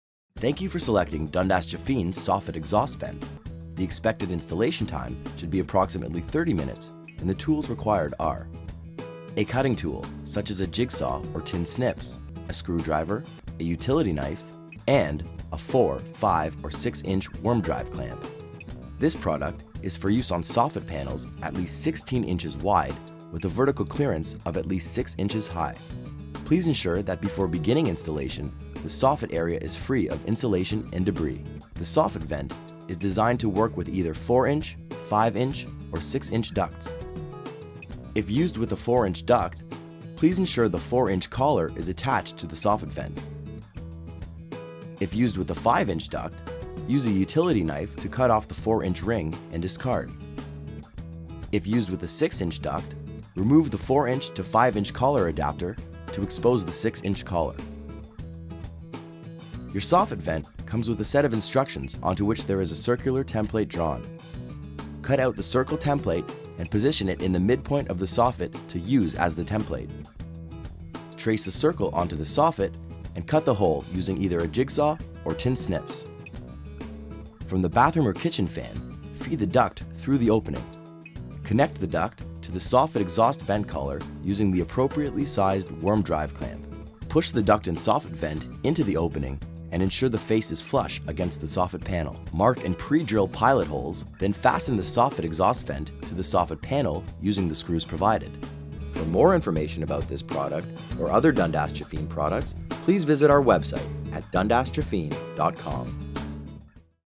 Soffit Exhaust Vent - Installation: Dundas Jafine
The Dundas Jafine soffit exhaust vent is ideal for exhausting bathroom and kitchen fans through the soffit of your home.  This product can be mounted under the soffit panel and connects to 4”, 5” or 6” ducts. Features include a frost free damper system that keeps cold air out and helps conserve energy. The stylish flush mount design can be painted to match the exterior of the home and the leak proof collar to duct connection helps prevent moist air backdraft.

This product has an outer grill and flap preventing pests from entering your home through the duct. It is designed to work on a minimum of 50 CFM fan, installed on a 25′ duct run.

Like many of our other products, the Soffit Exhaust Vent has a UV resistant coating to prevent the cap from discoloring and is paintable to match the soffit of your home.